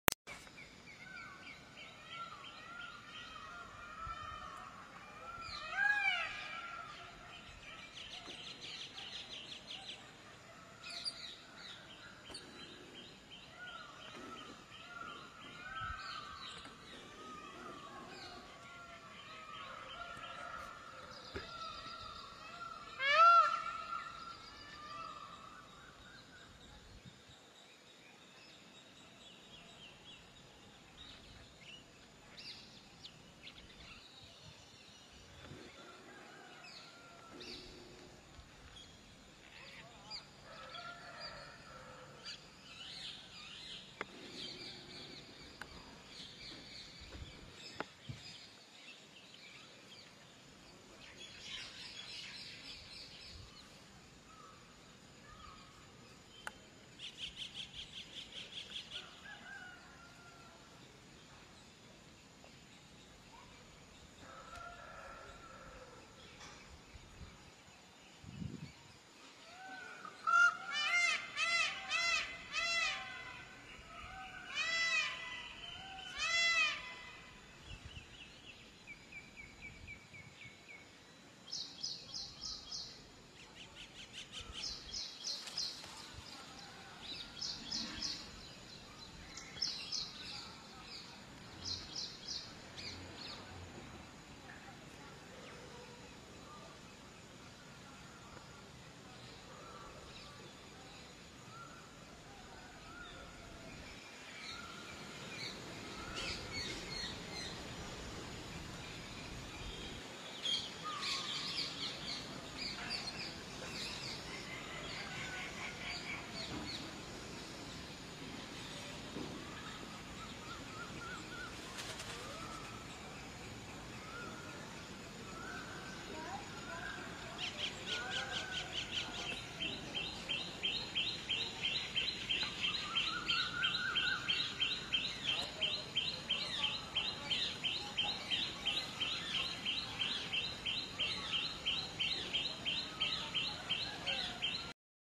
Peacock Dance Display_Peacock Opening Feathers Full HD &Birds sound_Peacock Video
Beautiful Peacock video & Dance This video ...

aise he funny video dekhne k liye Niche diye link par aap click kar sakte hain ..